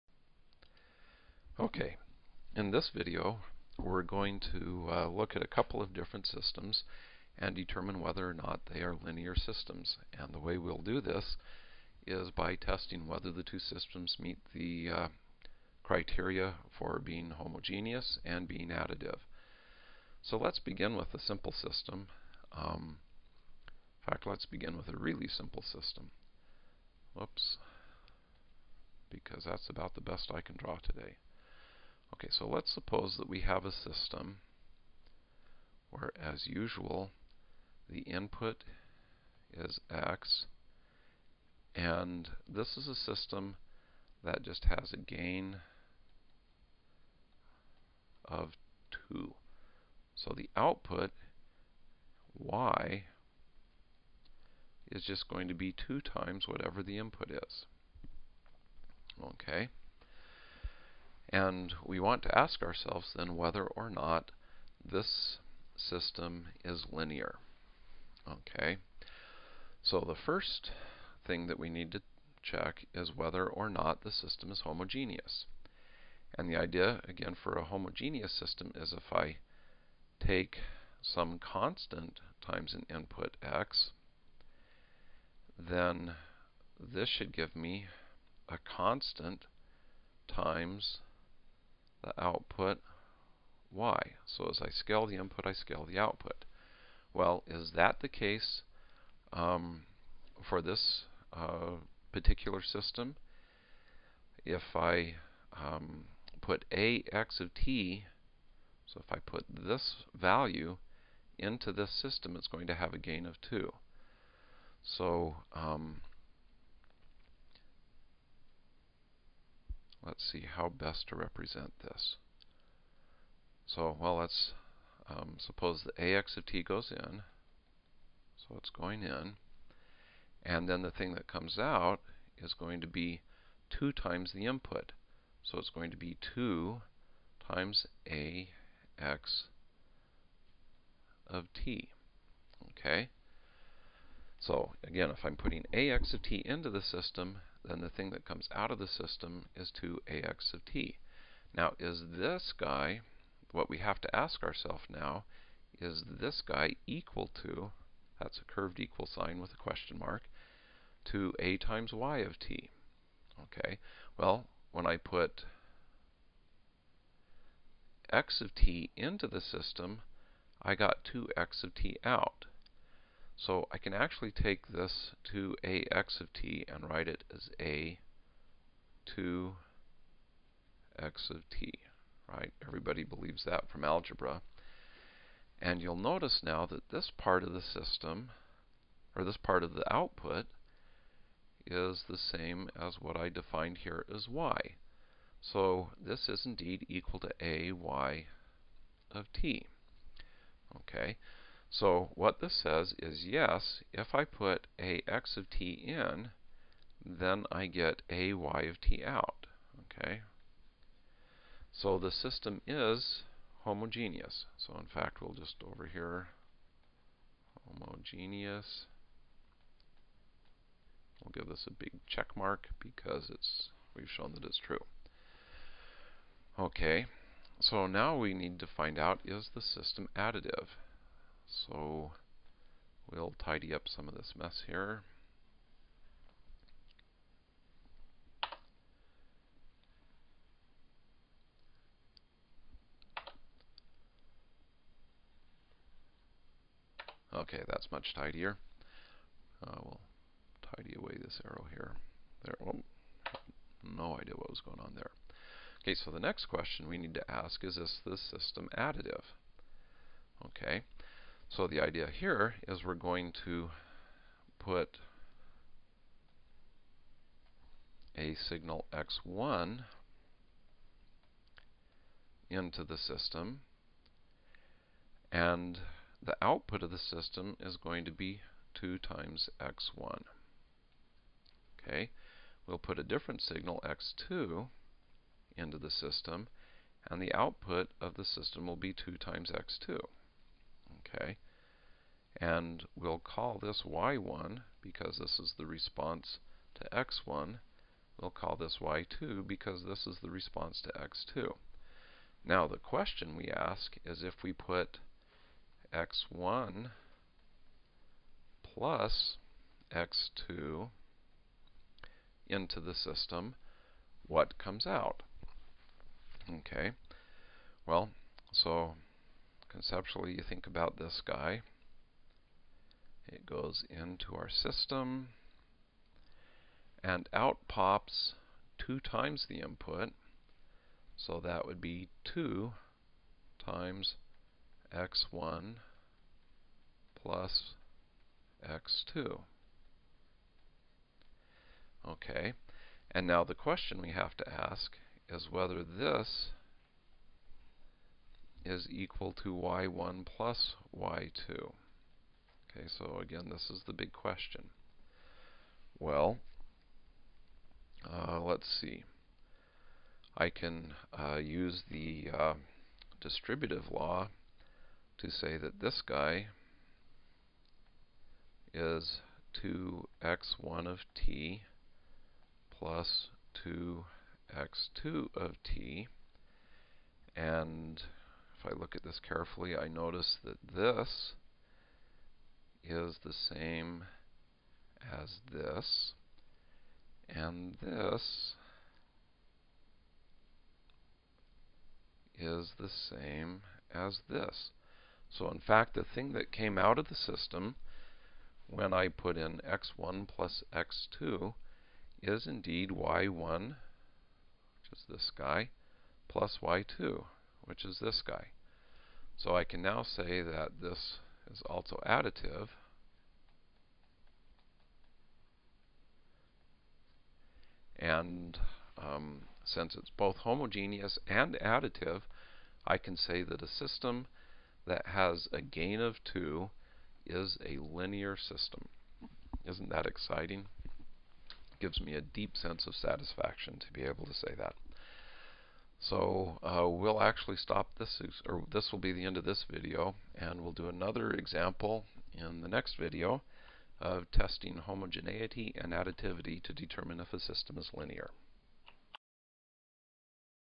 Linearity Example: Gain
An example of determining whether a gain (a system that scales the amplitude of a signal) is a linear system.

This video is one in a series of videos being created to support EGR 433:Transforms & Systems Modeling at Arizona State University. Links to the other videos can be found